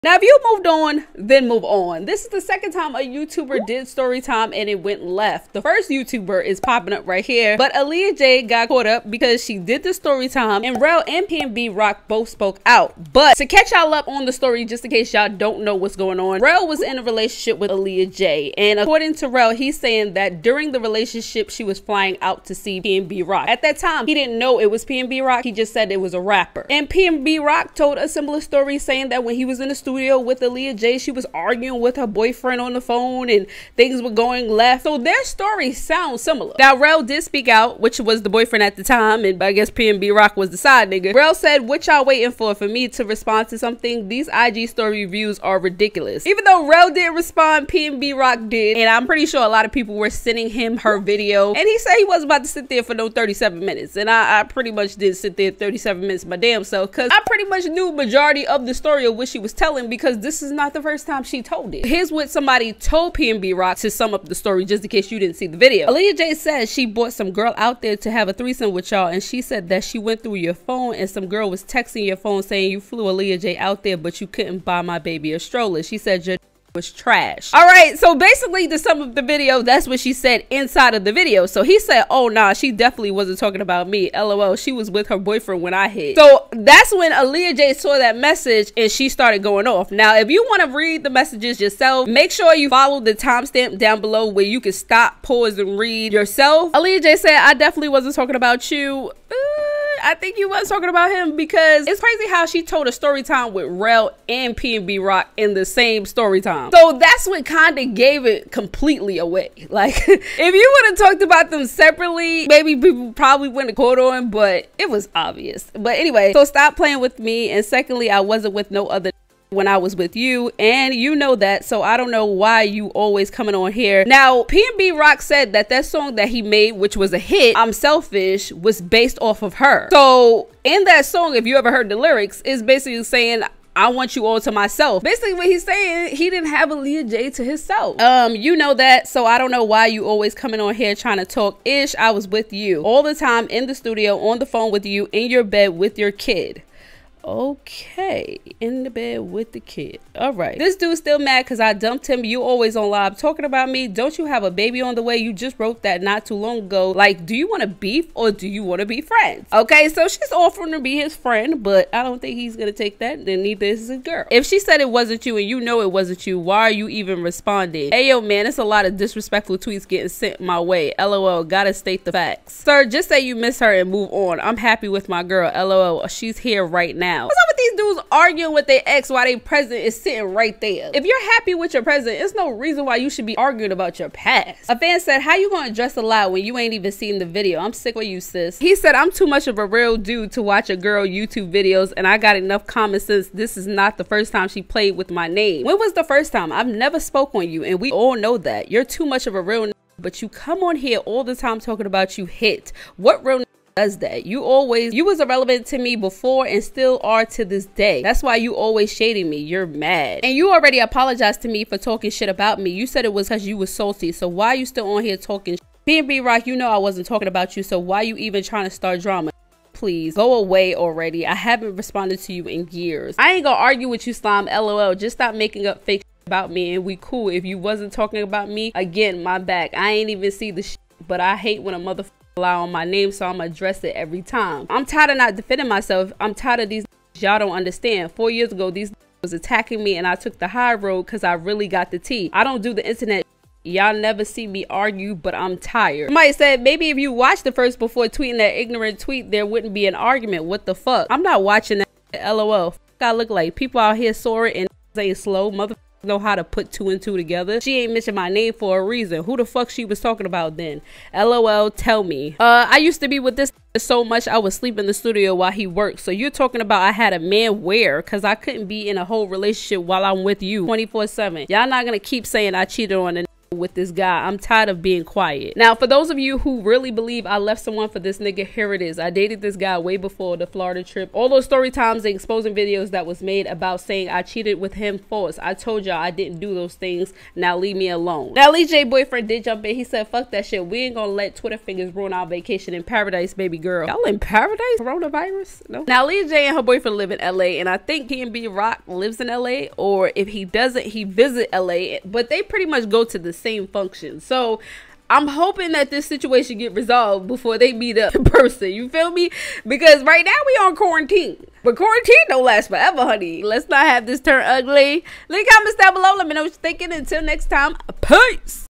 AaMsAaliyah Jay Went 0FF On EX PNB Rock After He Said 😳☕️ "I H!T While She H@d A Man
0:41 👉🏼Messages start (Aaliyah Jay Ex Rell) …
1:06 👉🏼PNB ROCK (Start)
1:37 👉🏼 PNB ROCK (2)
2:31  👉🏼 AALIYAH JAY CLAPBACK (3)
2:44 👉🏼PNBROCK (4)
2:53👉🏼 PNBROCK (5)
3:05 👉🏼 PNB ROCK (6)
3:16👉🏼 AALIYAH (7)
3:31 👉🏼AALIYAH (8)
3:44 👉🏼 PNBN ROCK (9)
3:55 👉🏼 AALIYAH (10)
4:06 👉🏼 PNB ROCK (11)
4:23 👉🏼 AALIYAH (12)
4:36 👉🏼 AALIYAH (13)

( FOR A SHOUT OUT PLEASE SAY “SHOUT OUT”
WITH SENT RECEIPTS OR I WILL ASSUME YOU DON’T

Celebrity Updates Early 👇🏽

Thevoice4dappl